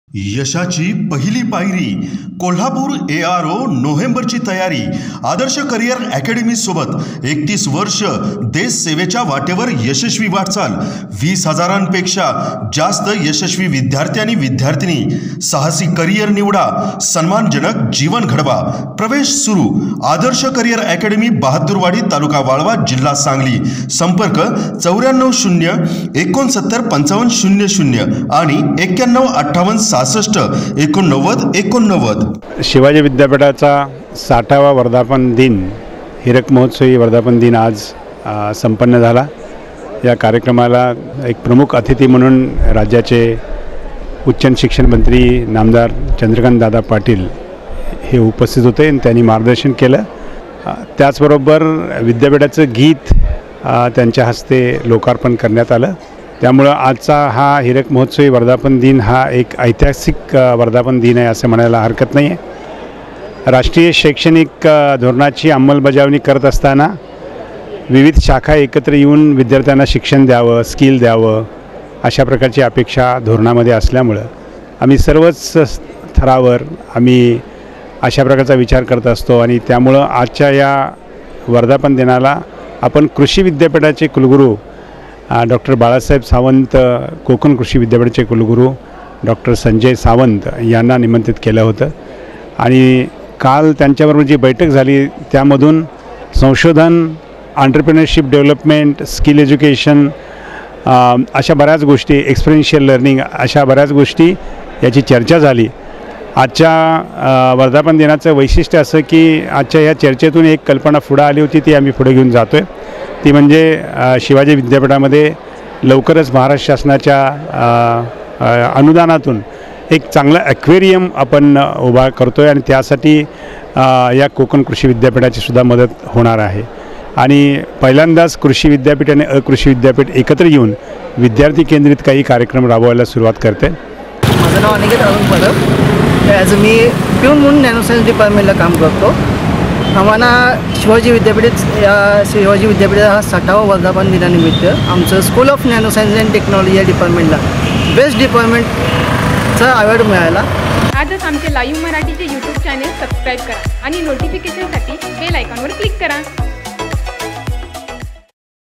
लवकरच शिवाजी विद्यापीठात मत्स्यालय उभारणार : डॉ. डी. टी. शिर्के
शिवाजी विद्यापीठाचा ६० वा वर्धापनदिन मोठ्या उत्साहात संपन्न झाला.